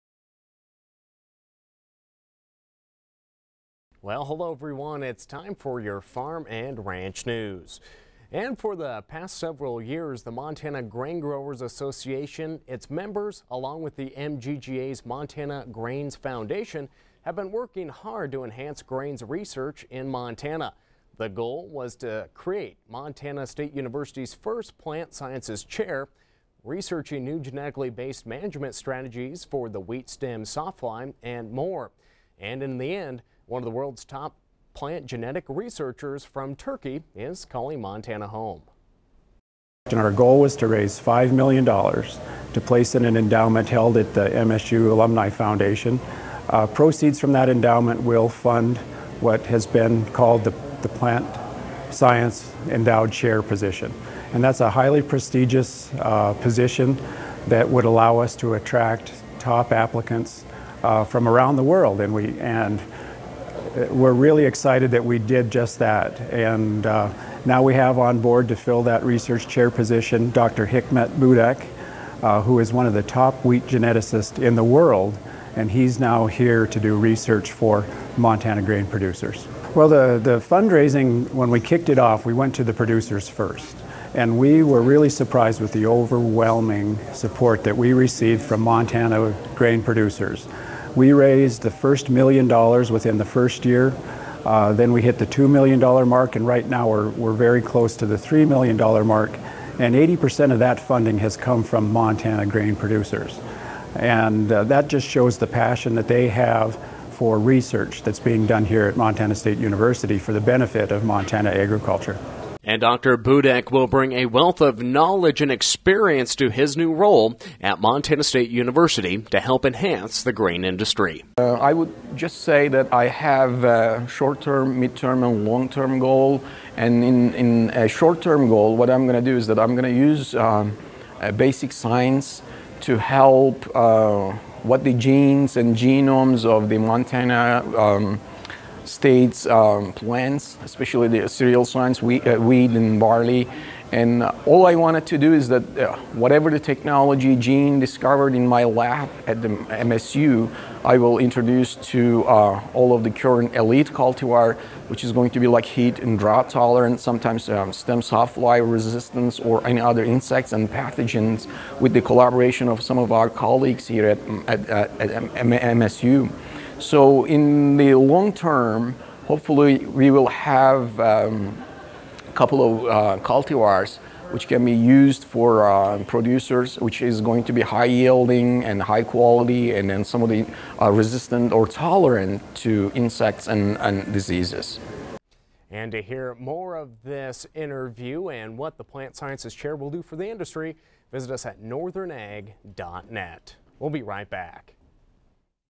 Montana Plant Sciences Chair
The head of a plant genetics and genomics team at an international university who is considered a world leader in cereal genetics has been chosen as Montana State University’s first Montana Plant Sciences Chair.

Hikmet Budak from Sabanci University in Istanbul, Turkey, was selected for the position after a national search. He will begin at MSU in January.